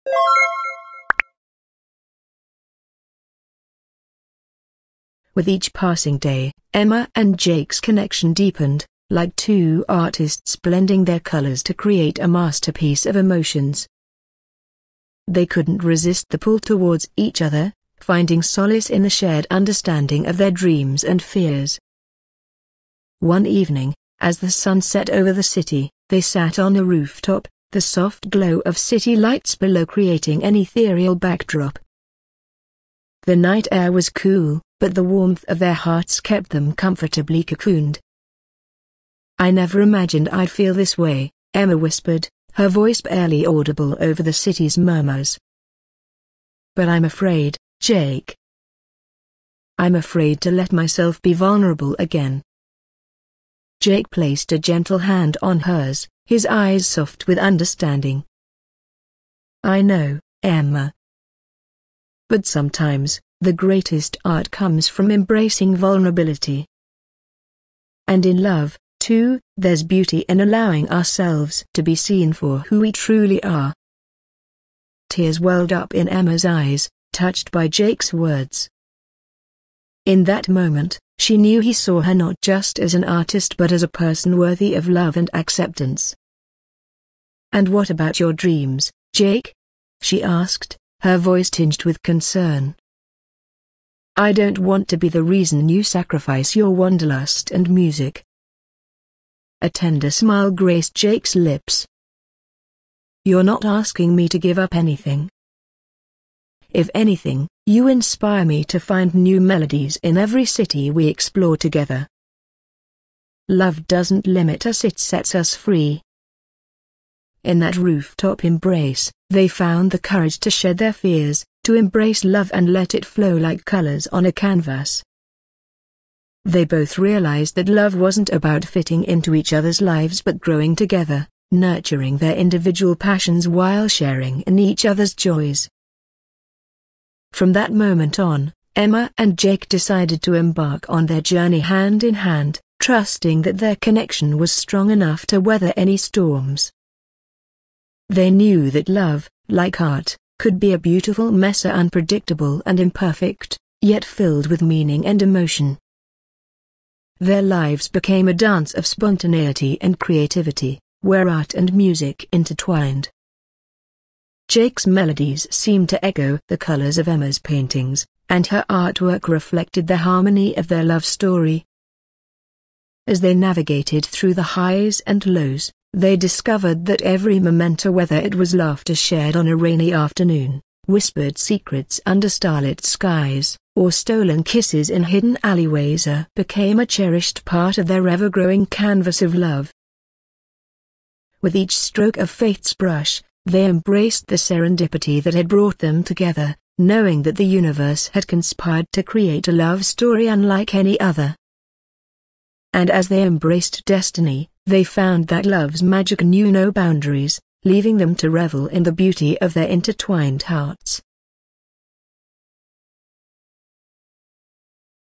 Serendipity of Love | Chapter 3: Embracing Destiny. (Audio Story)
Amidst the enchanting city lights, Emma and Jake find solace on a rooftop, baring their hearts and insecurities. Their fears of vulnerability dissipate as they realize the beauty of being seen and loved for who they truly are. Embracing their individual dreams, they discover that love isn't about sacrificing, but inspiring each other. Their lives intertwine like art and music, with every moment becoming a cherished stroke on their canvas of love. In the serendipity of their connection, they dance freely, trusting that destiny has carefully woven their paths together, creating a love story that defies expectations and embraces the magic of the unknown.